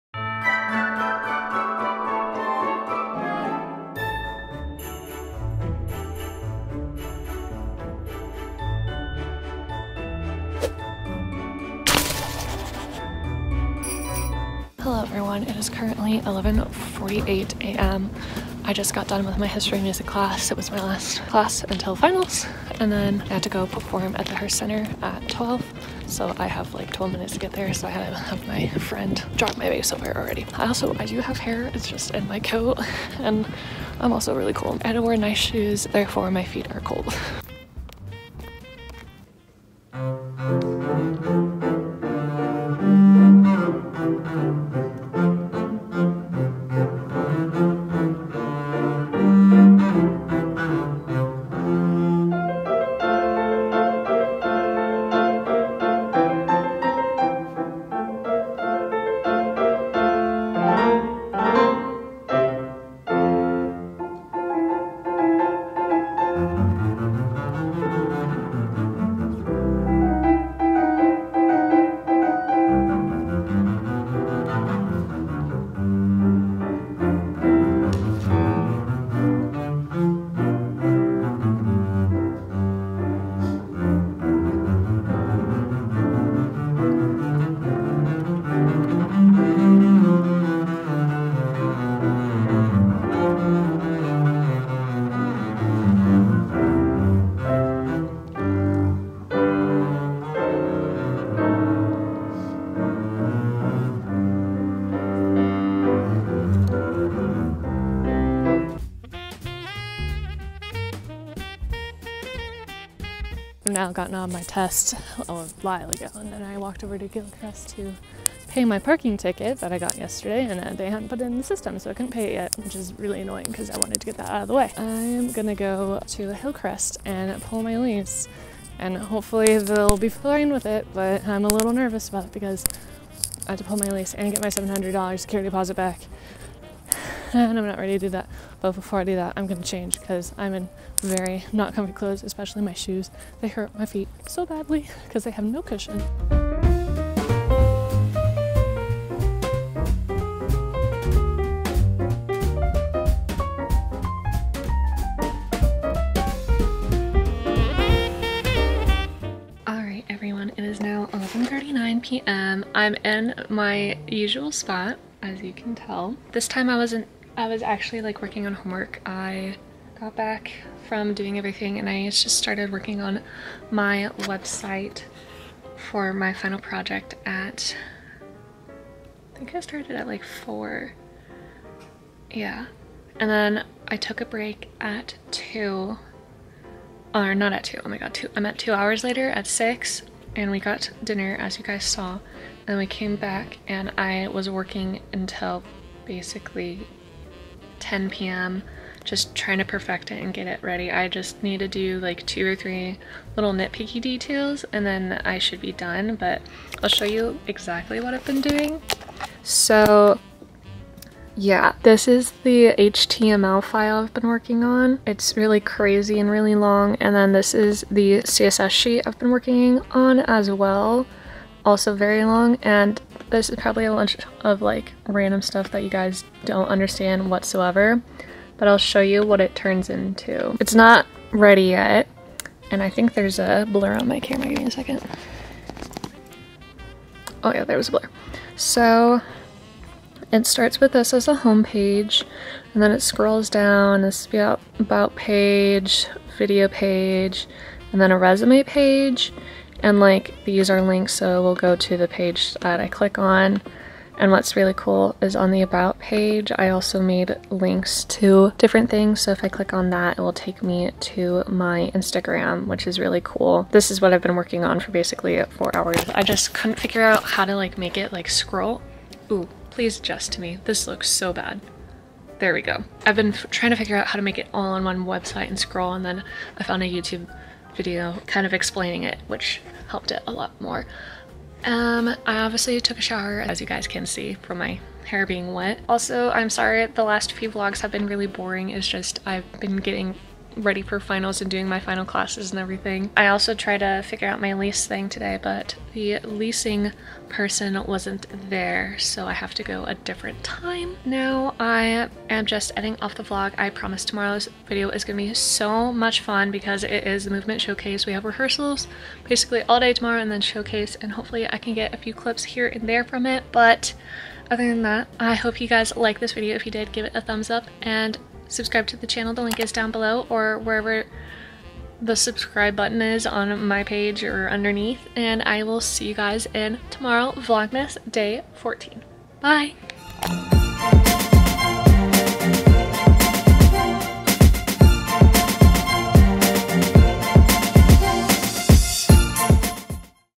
Making My Own Website | Vlogmas Day 13
Hello everyone! In today's Vlogmas, I went to class and worked on my final project which is creating my own website from scratch.  I hope you liked the video! See you soon :)
xoxo, Catherine